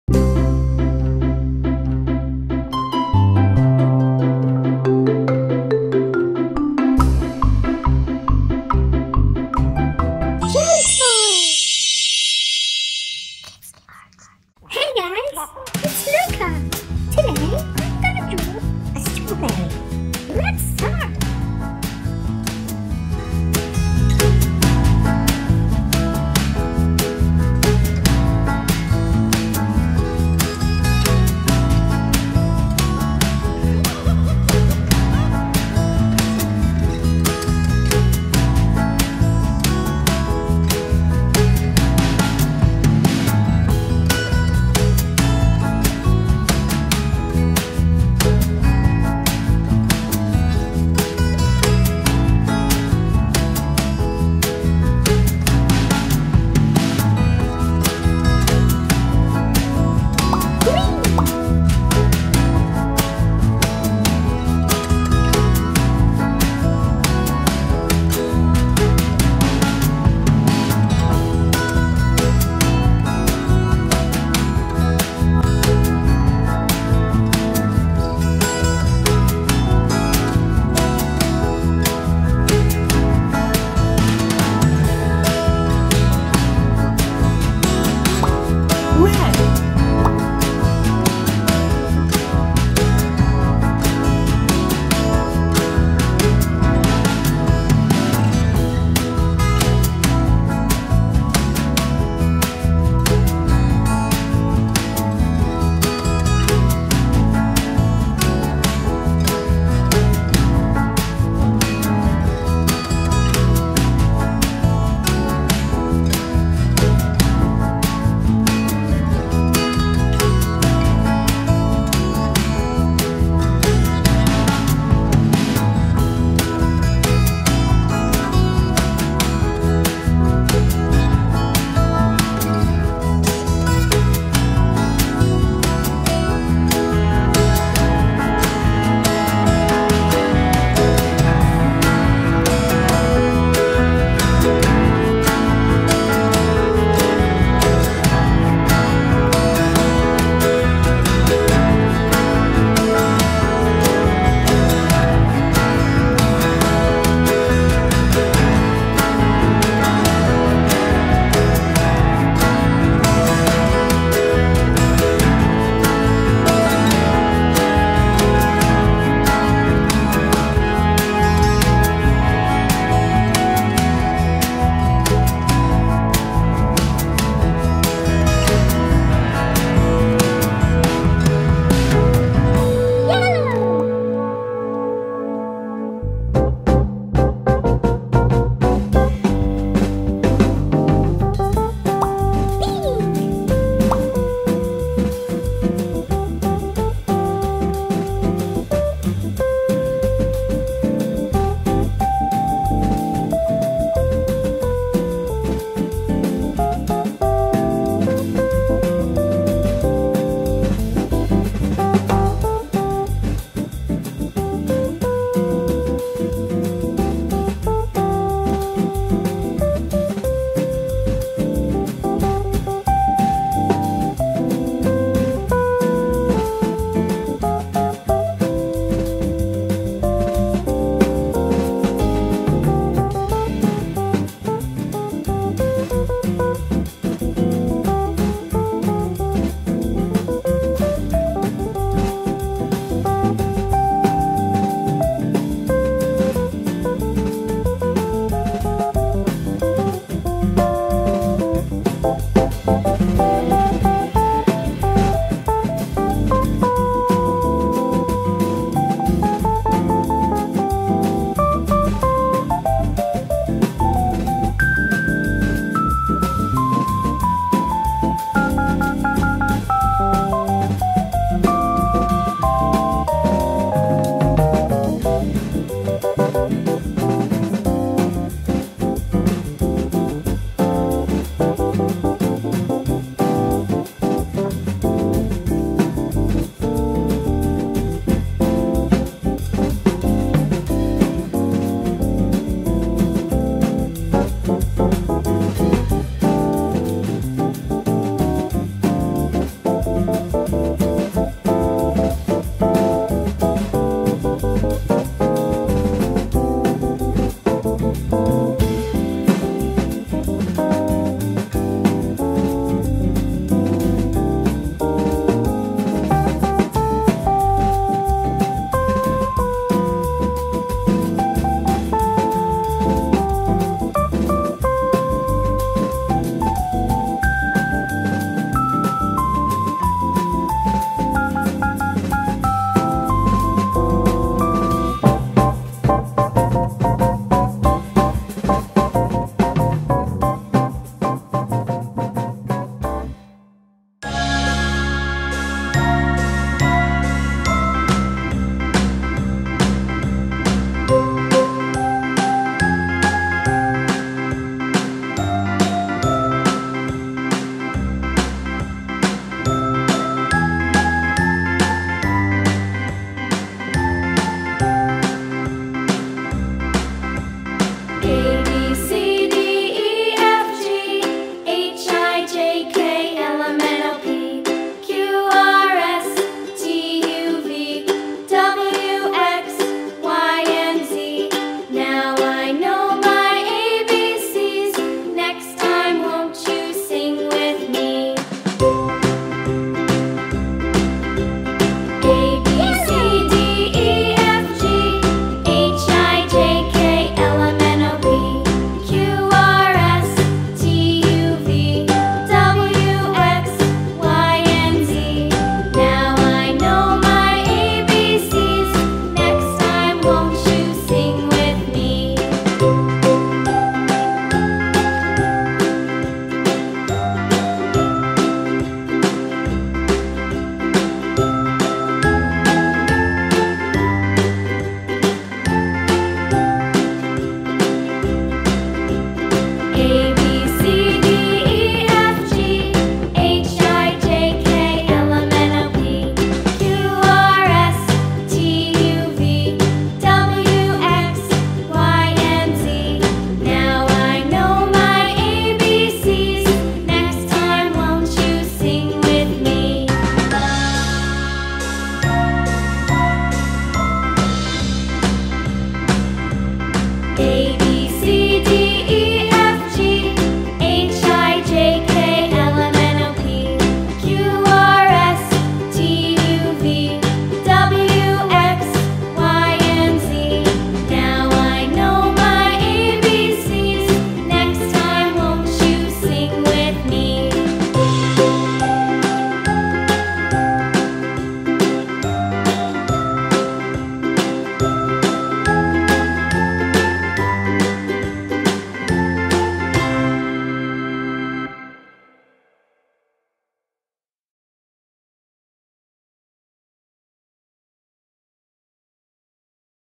Menggambar Dan Mewarnai Stroberi Pelangi dan Buah-buahan Untuk Anak-anak
Belajar Menggambar Dan Mewarnai Untuk Anak-anak
JELLY TOY Art ♡
شكرا للمشاهدة ♡
♡ جيلي لعبة أطفال الفن ♡